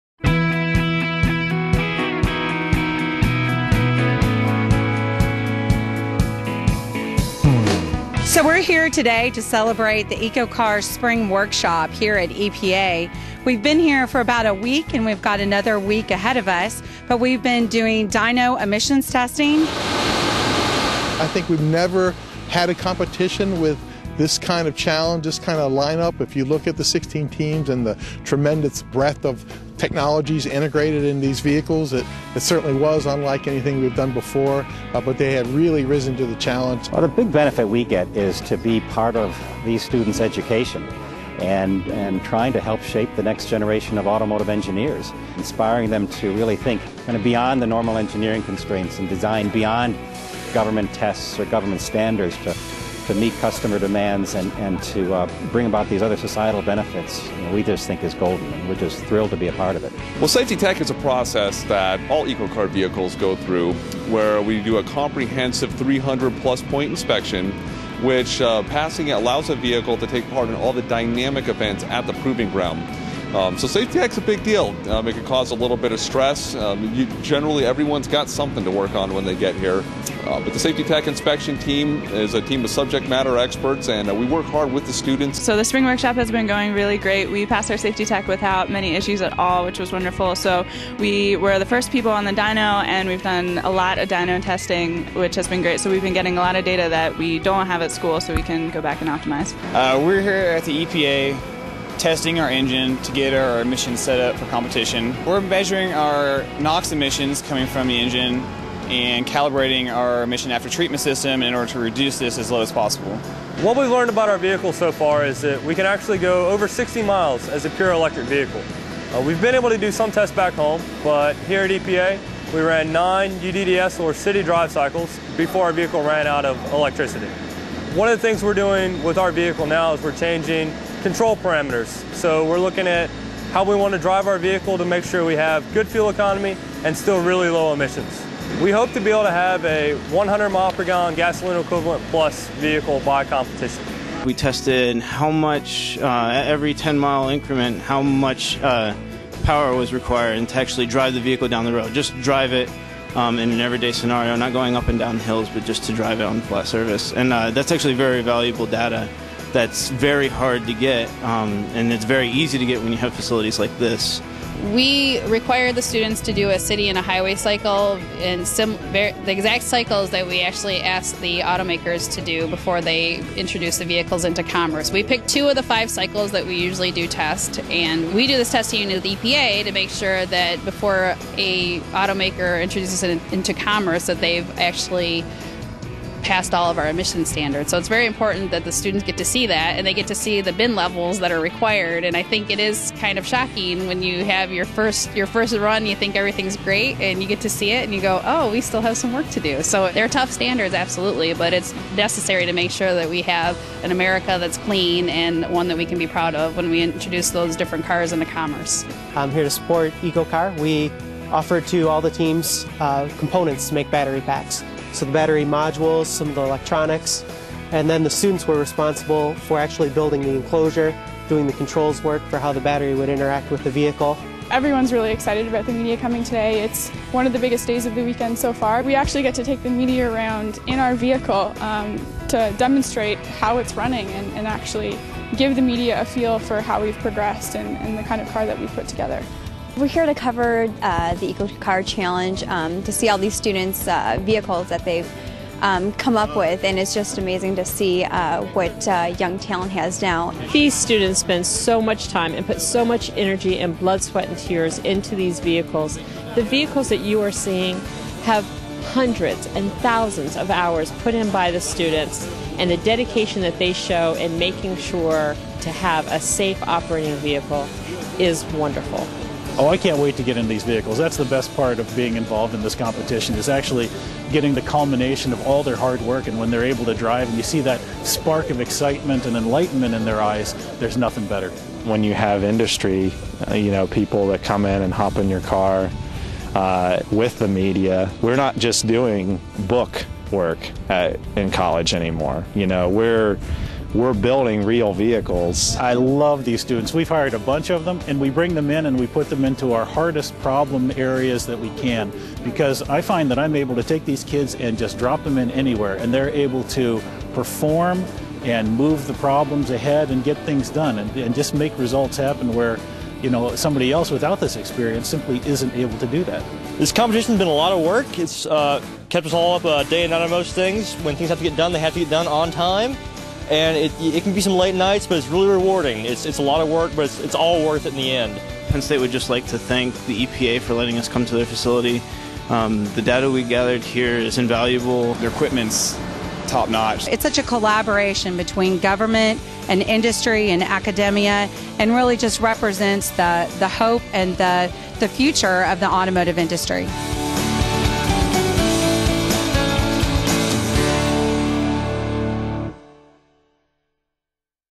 2011 Spring Workshop at EPA
The EcoCAR Year Three Spring Workshop took place at the Environmental Protection Agency's (EPA) National Vehicle and Fuel Emissions Laboratory, where teams took part in safety tech inspections and begin dynamometer and emissions testing on their vehicles.